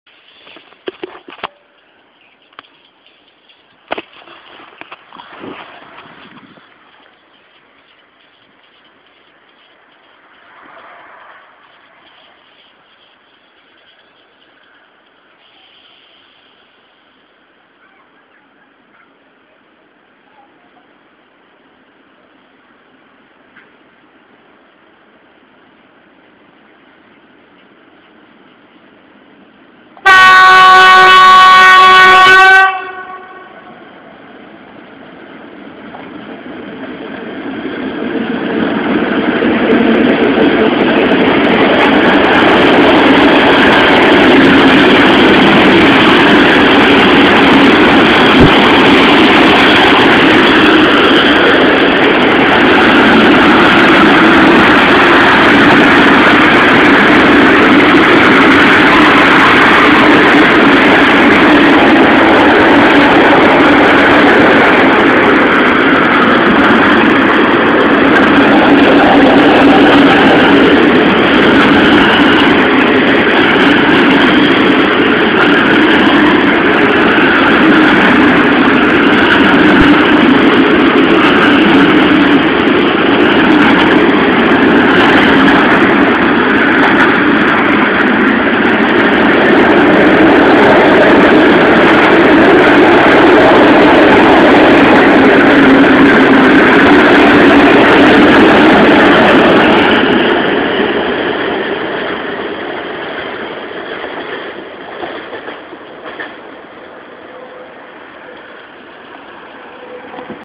PKP = TGV
Super Szybki Pociag sfilmowany w Czestochowie. Predkosc maksymalna nieprzekracza 50km/h